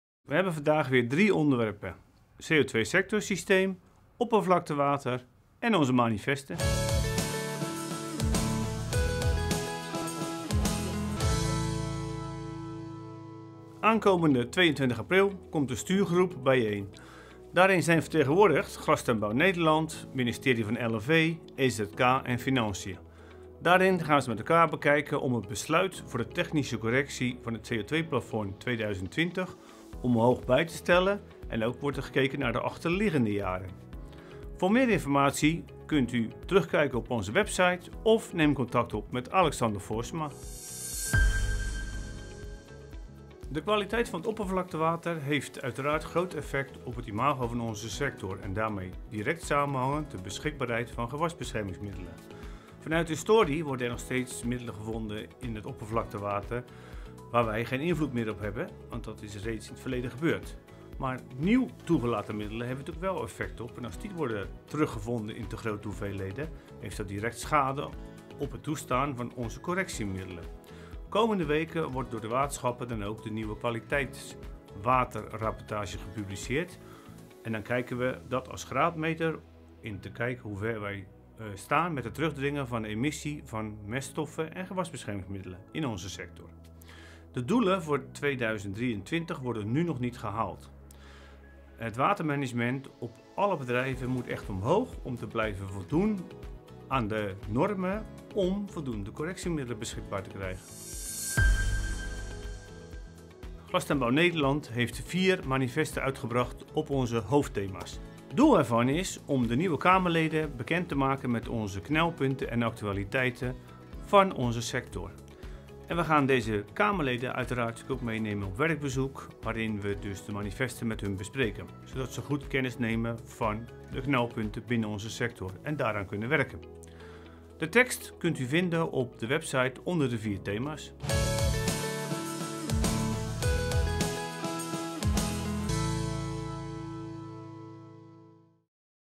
Kas Topics: CO2, oppervlaktewater en manifesten
In deze KasTopics bespreekt directeur Ruud Paauwe drie onderwerpen: de bijeenkomst van de stuurgroep CO2 Sectorsysteem, de waterkwaliteitsrapportages en daaraan gekoppeld de prestaties van de glastuinbouw en als afsluiting de manifesten van de hoofdthema's van Glastuinbouw Nederland voor de politiek.